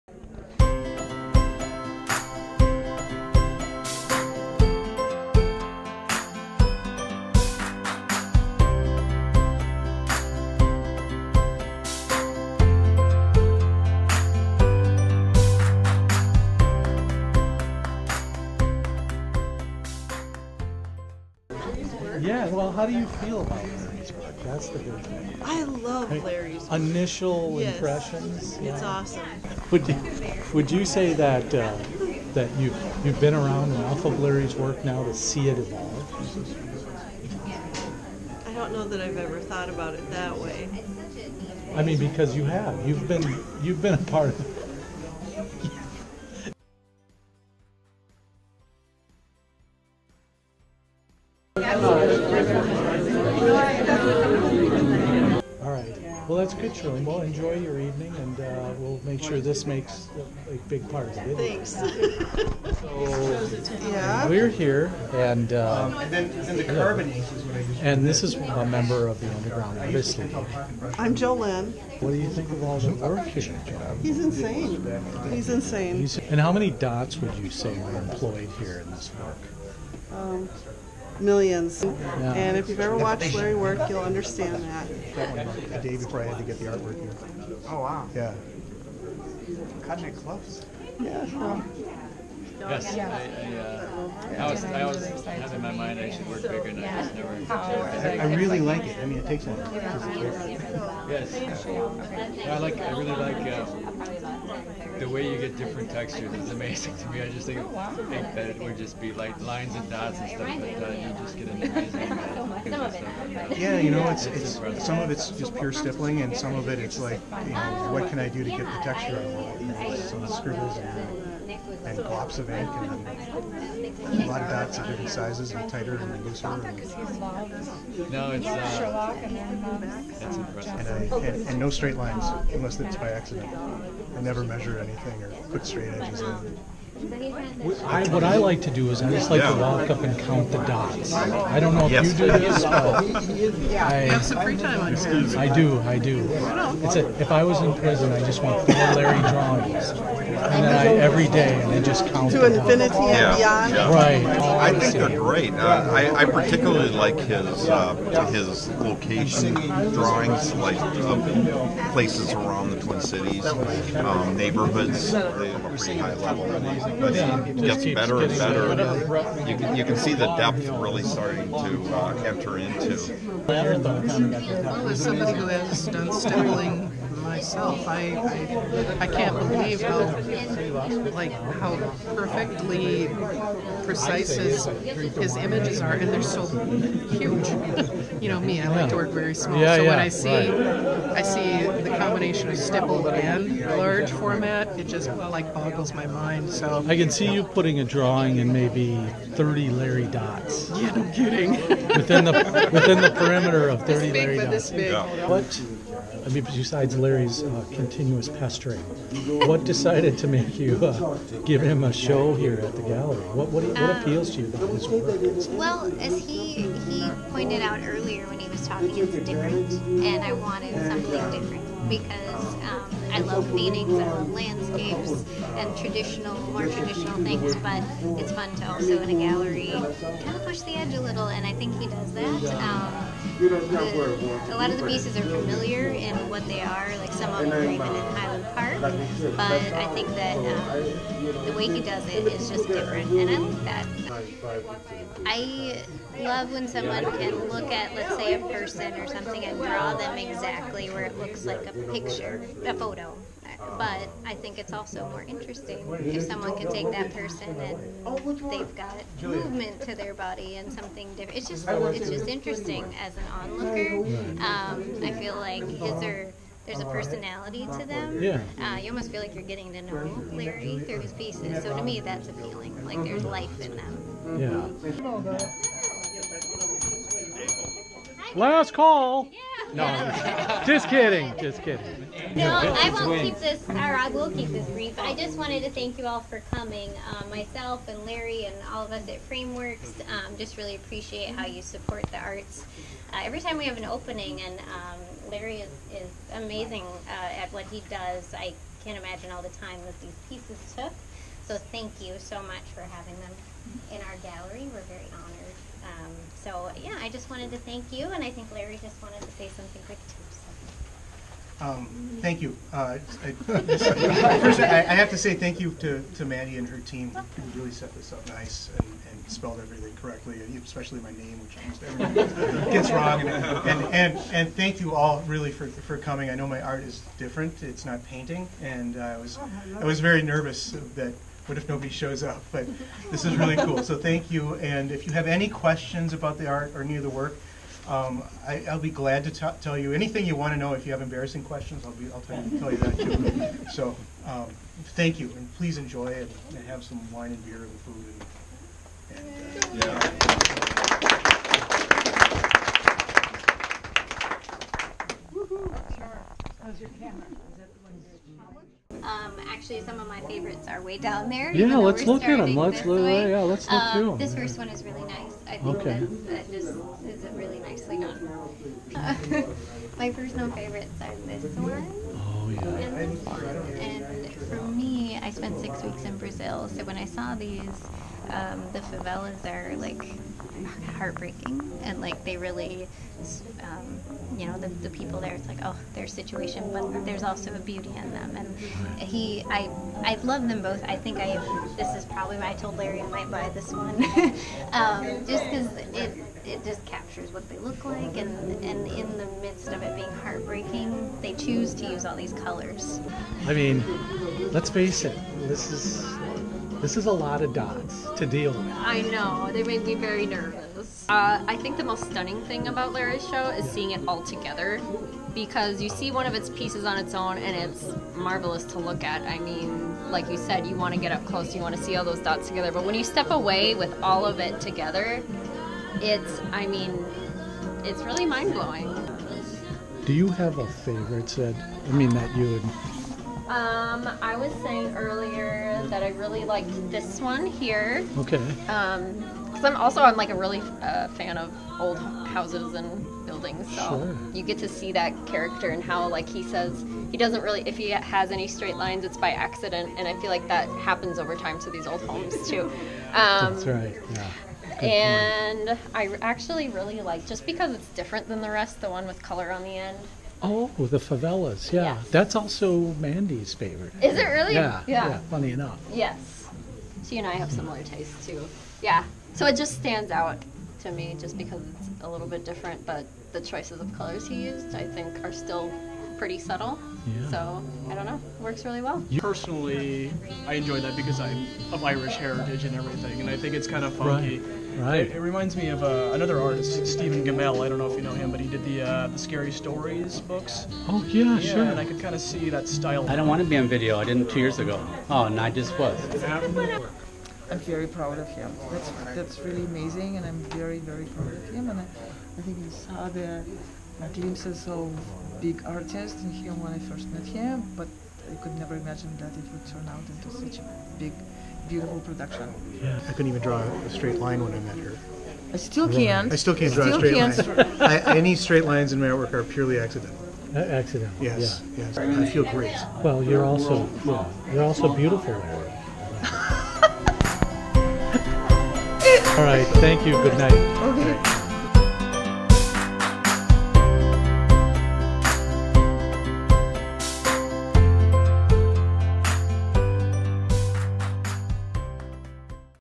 Art Opening - A Feast Of Autumn
A documentary film of Larry Ehrlich's first solo art opening. At the Frameworks Art Gallery in Saint Paul, Minnesota on the Saturday evening November 4, 2018.

Special thanks to The Frameworks Gallery.